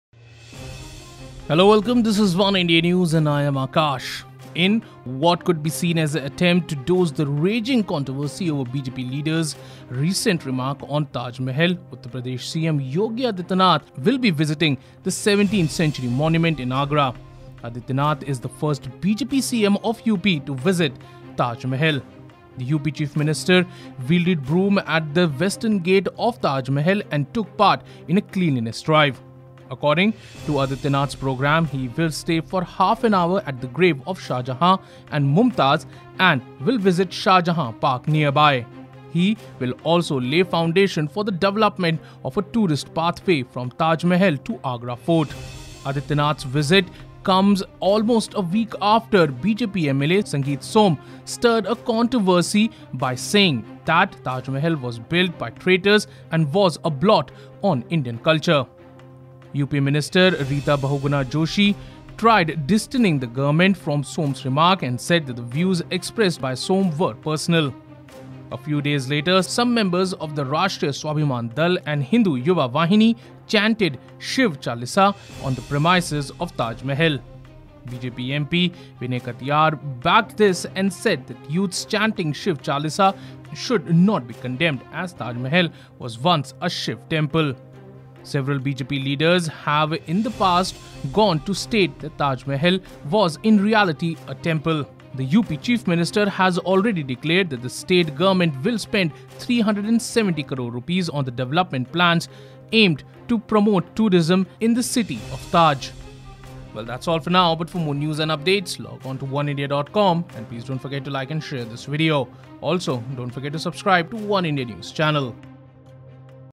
Yogi Adityanath to visit Taj Mahal as BJP looks for damage control | Oneindia News
In what can be seen as an attempt douse the raging controversy over BJP leaders' recent remarks on Taj Mahal, Uttar Pradesh Chief Minister Yogi Adityanath on Thursday visited the 17th-century monument in Agra. Adityanath is the first BJP CM of UP to visit the Taj Mahal. The UP Chief Minister weilded broom at the Western Gate of Taj Mahal and took part in a cleanliness drive. According to Adityanath's programme, he will stay for half an hour at the graves of Shah Jahan and Mumtaz Mahal and visit the Shah Jahan park nearby. He will also lay foundation for the development of a tourist pathway from Taj Mahal to Agra Fort. His visit comes almost a week after BJP MLA Sangeet Som stirred a controversy by saying that the Taj Mahal was built by traitors and was a 'blot' on Indian culture.